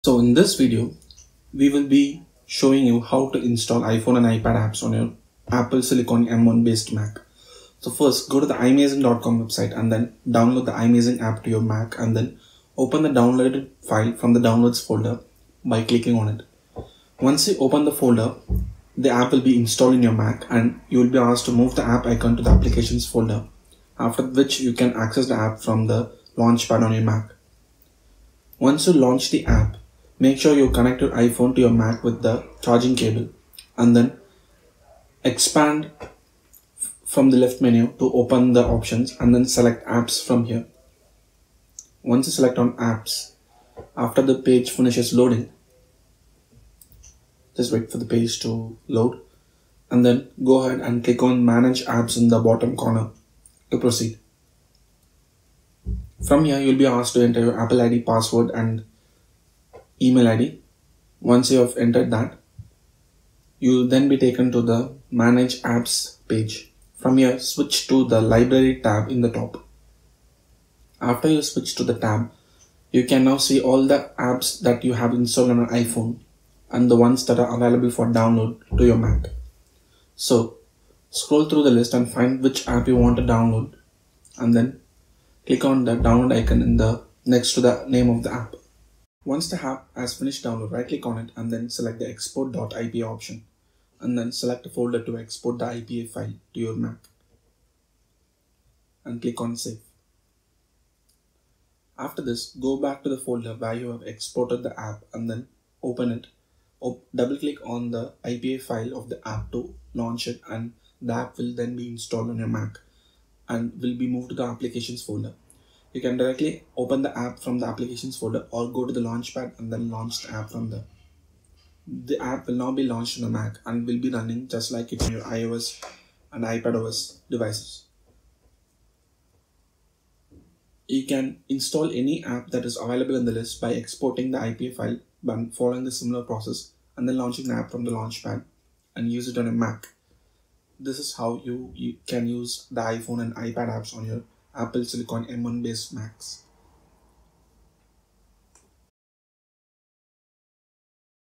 How to Install iPhone and iPad apps on Apple M1 Mac 2020
If you recently bought an Apple M1 based Macbook or Mac mini, then you might be knowing that you can now run iPhone and iPad apps on your M1 based Macs. However, not all apps are natively available from the App store. But, fortunately, there is a workaround to install iOS and iPadOS apps on your macOS device. Watch the video to know how to!